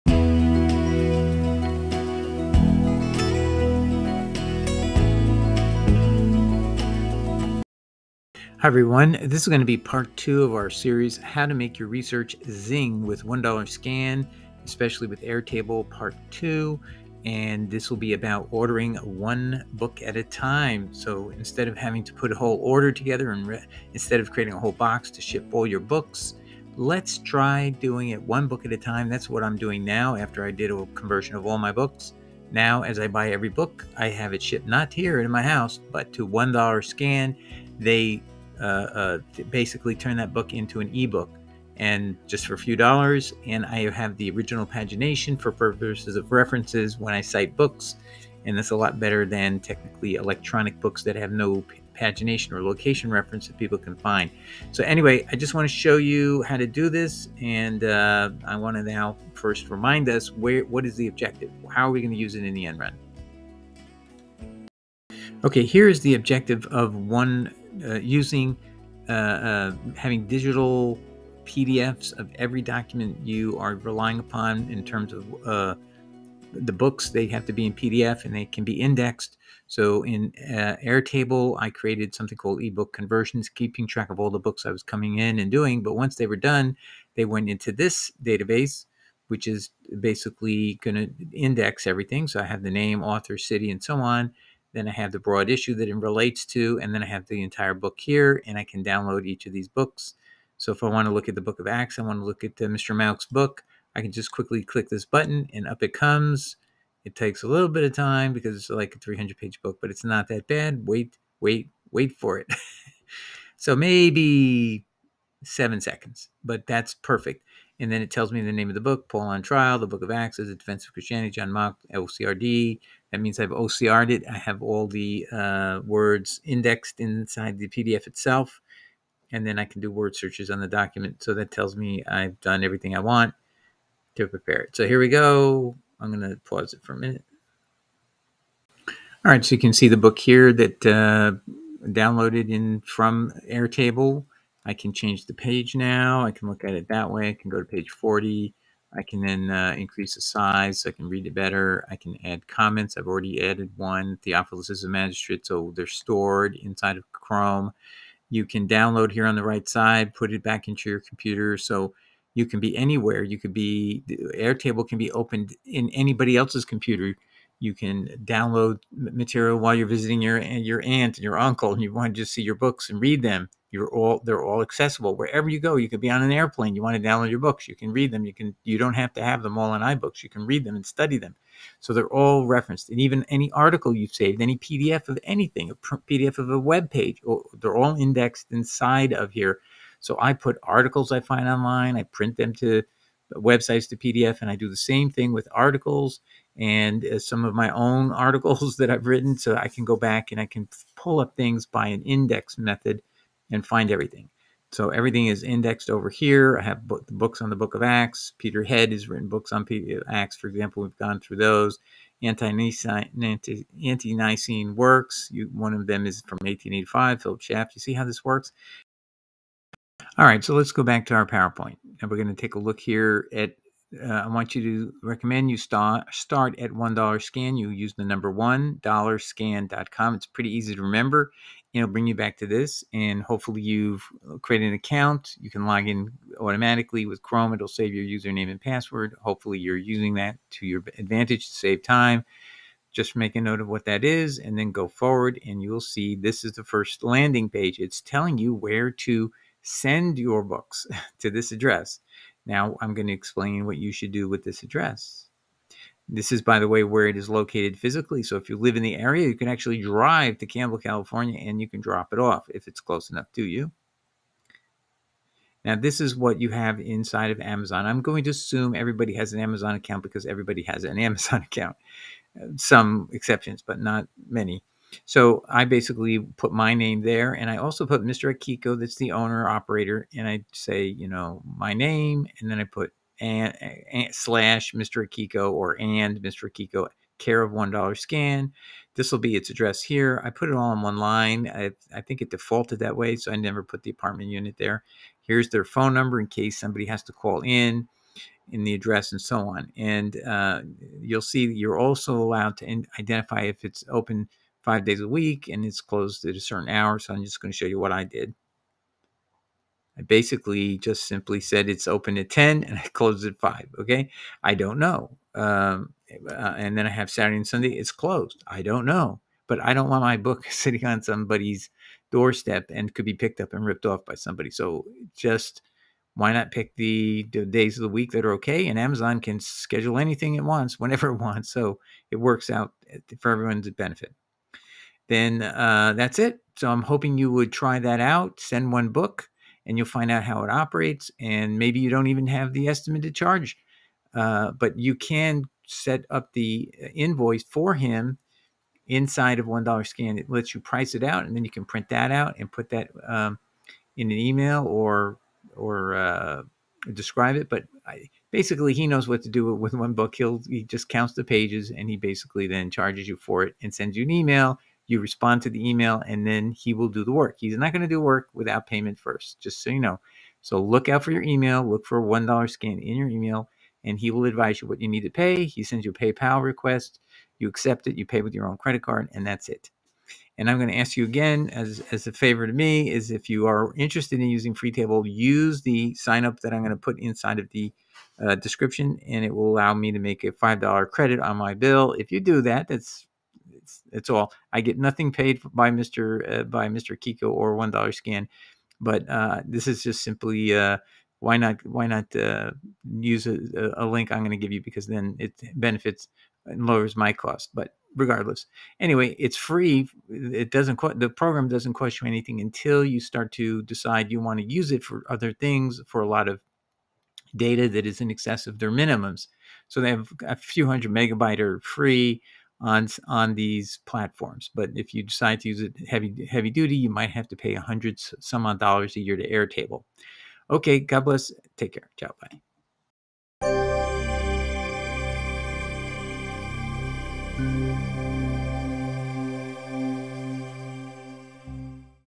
$1 Scan part 2 - How to Order 1 Book at a Time to be Converted into A PDF.
Please sign up for a free Airtable account to use for researches and study -- the best app to keep notes as well as store all your books.